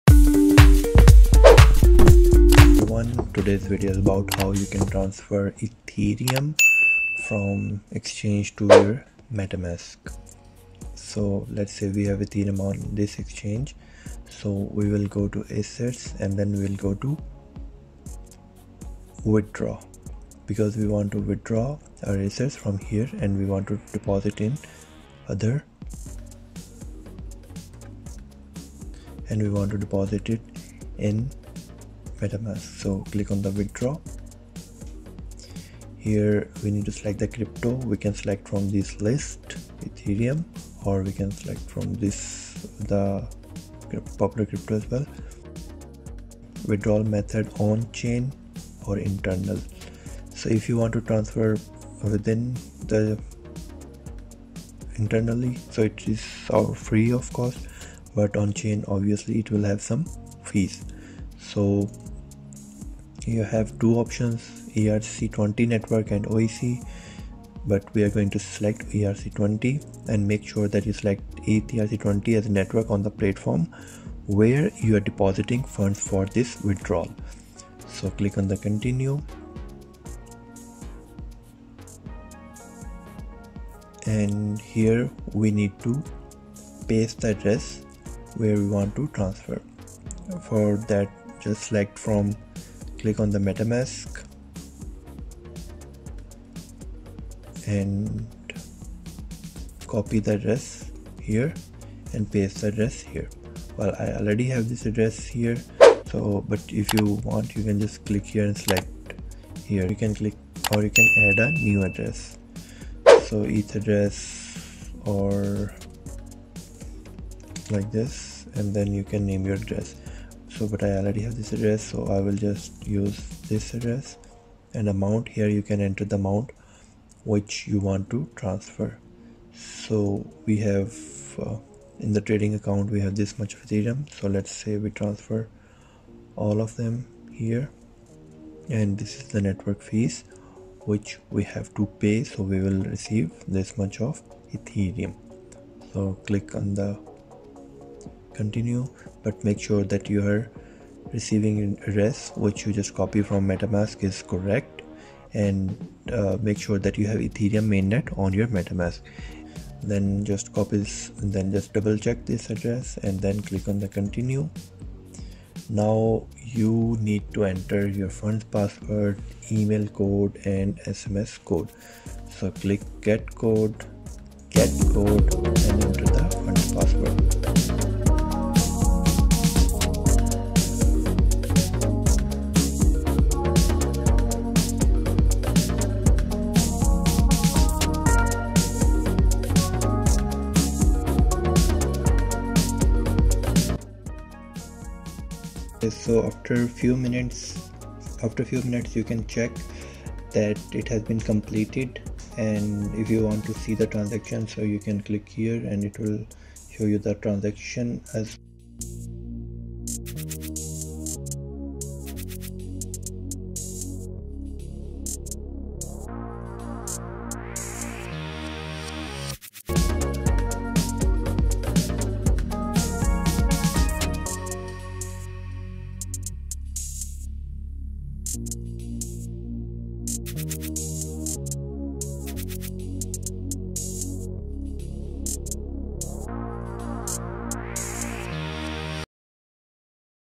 How to Transfer Ethereum (ETH) from OKX (OKEX) to Metamask
This video is about you can transfer Ethereum from exchange to Metamask. OKEX has rebranded as OKX.

BSC (BEP20) Network for USDT Wallet: 0x1b1afd3eca51ed532107c2a06443f4ec5395d785

Timestamps:
00:00 Intro
00:13 Withdraw option on OKX
00:45 Select chain to withdraw from OKX
01:31  Withdrawl address from Metamask
02:46 Funds password on OKX
03:30 COnfirm ETH on Metamask
03:40 Outro